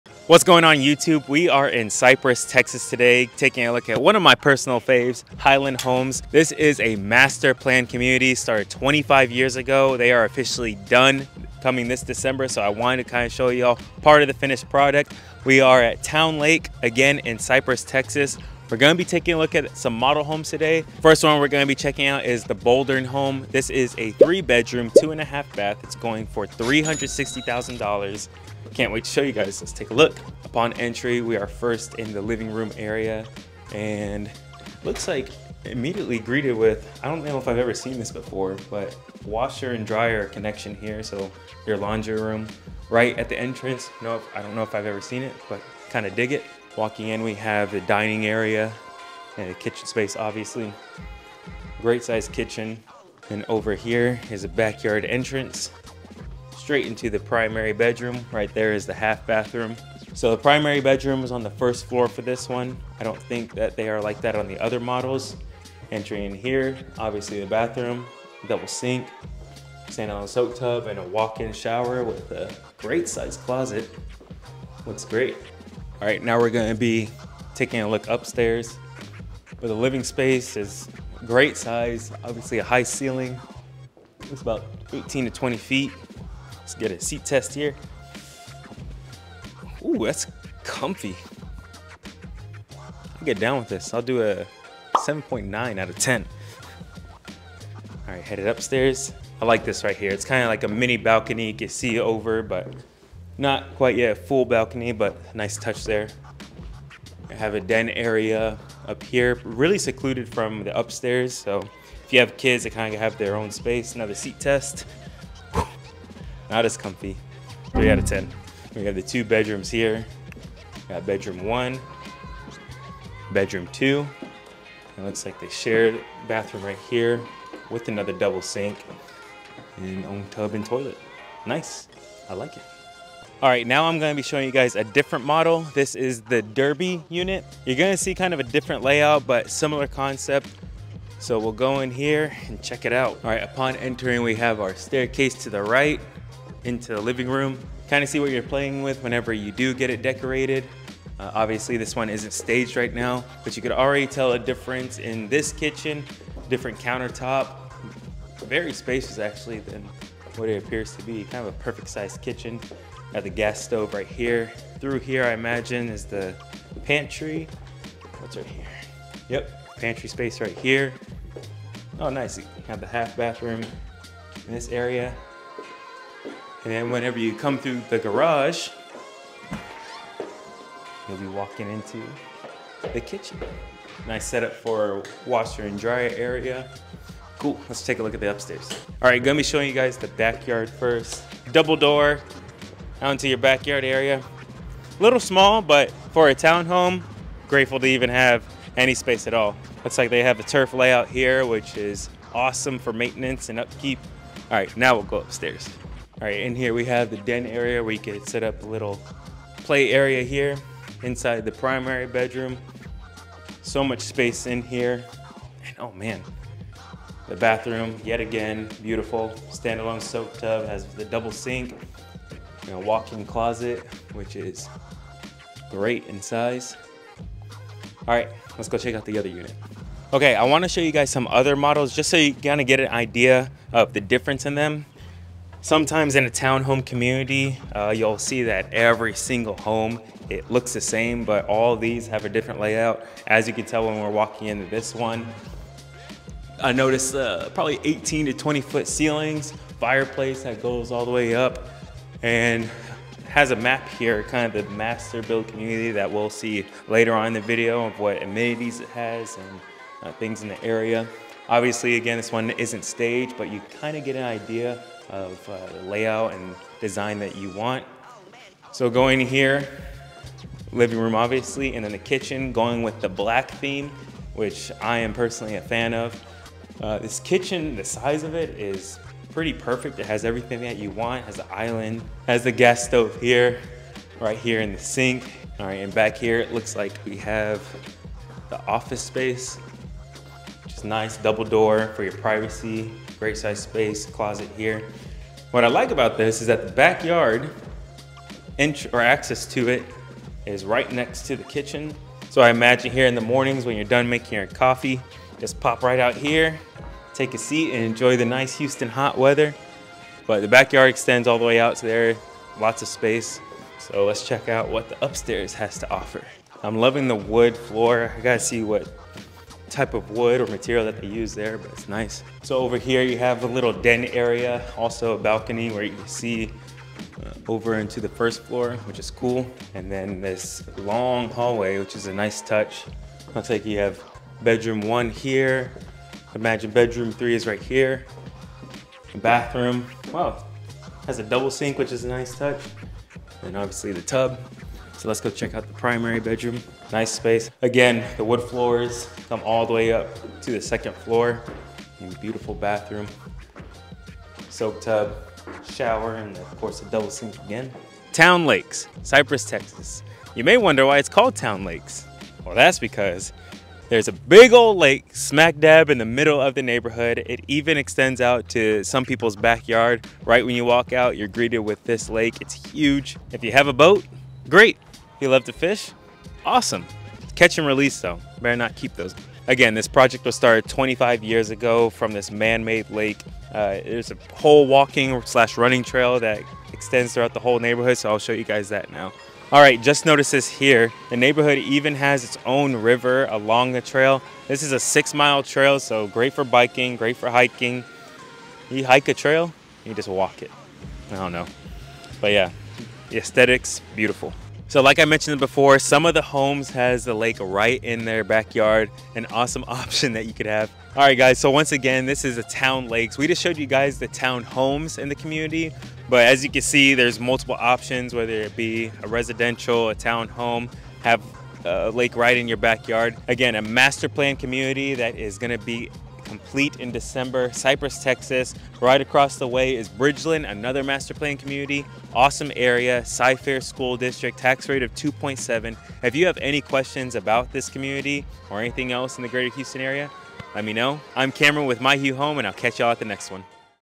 20 YEARS to Complete This Community in Cypress, TX! 😱
20 YEARS to Complete This Community in Cypress, TX! 😱

Welcome to Towne Lake, Cypress, TX’s premier master-planned community that’s been 20 years in the making! 🏡✨ This week, we’re diving into the stunning Towne Lake, a community designed for the ultimate lifestyle experience. Nearly two decades of thoughtful planning and development have culminated in a neighborhood that offers unparalleled amenities and beautiful homes.

Join me as we tour the exquisite Highland Homes, starting at $360,000. These homes offer a perfect blend of luxury and comfort, designed to meet the needs of modern families. From the state-of-the-art amenities to the scenic lake views, Towne Lake has something for everyone.

Don't miss out on seeing why Towne Lake is one of the most sought-after communities in Cypress.